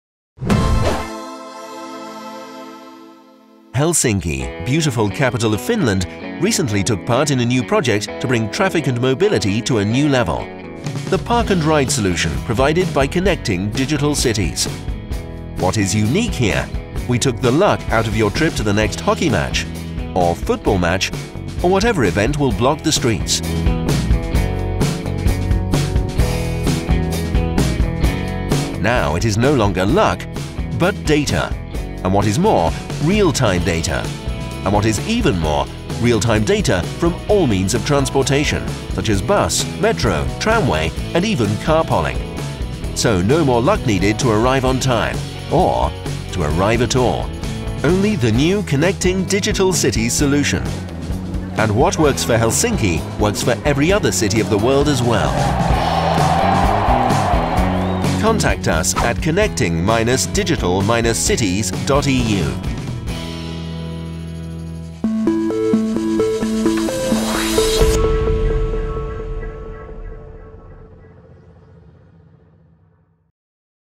Connecting Digital Cities - Evolving mobility - Helsinki
Connecting Digital Cities solution is an application to provide vehicle drivers and travellers with smart and real-time adjusted inter-modal itineraries, based on crowd sourcing and mobility analytics services.

Connecting Digital Cities has been supported by EIT Digital as an innovation activity in the Urban Life & Mobility action line.